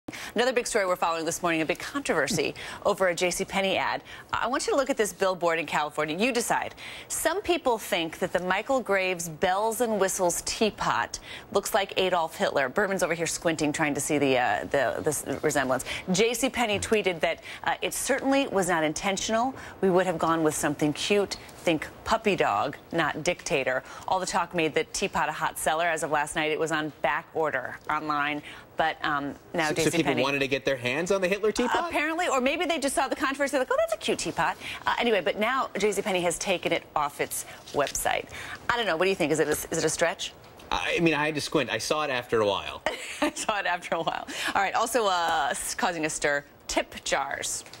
Tea pot that resembles Hitler? (May 29, 2013)
Tea pot that resembles Adolf Hitler? Controversy about J.C. Penny's California billboard advertising tea pot. J.C. Penny says it was "unintentional". Wednesday, May 29, 2013.